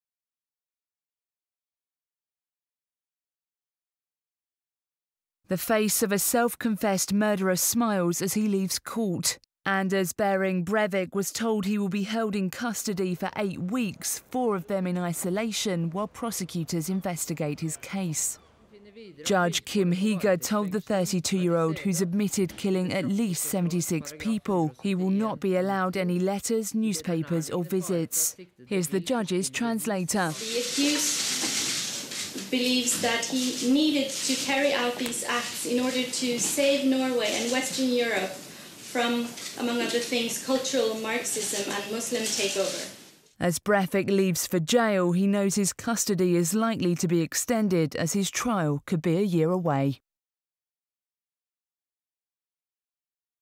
Norway killer will spend a month in isolation
Court rules for Anders Behring Breivik to stay in custody and spend four weeks in isolation.